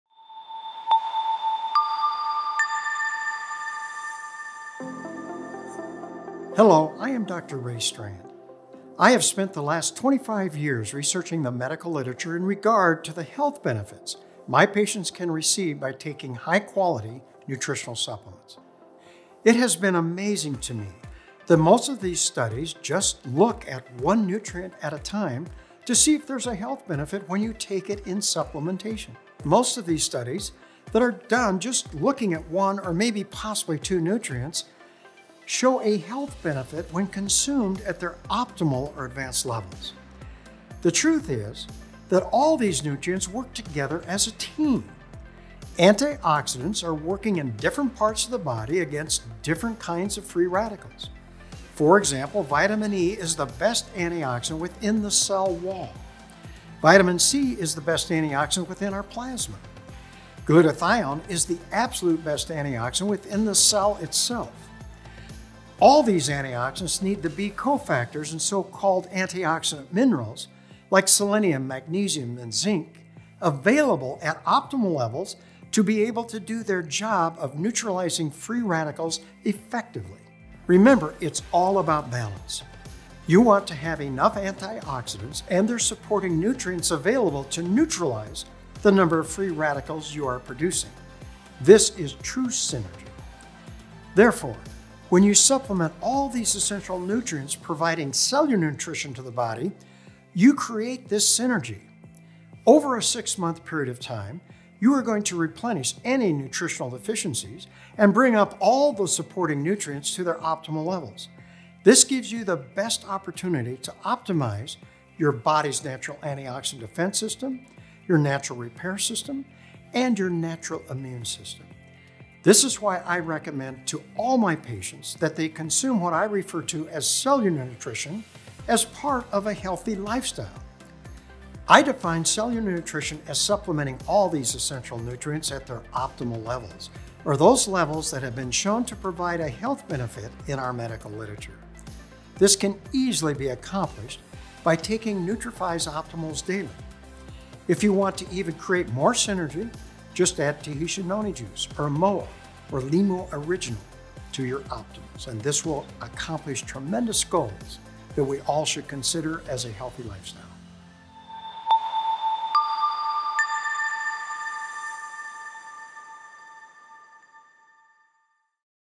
The Synergy of Supplements
Learn from Ray Strand, MD how supplements, when used at their optimal levels, work together with nutrients and antioxidants to support cellular health and help achieve synergy within the body.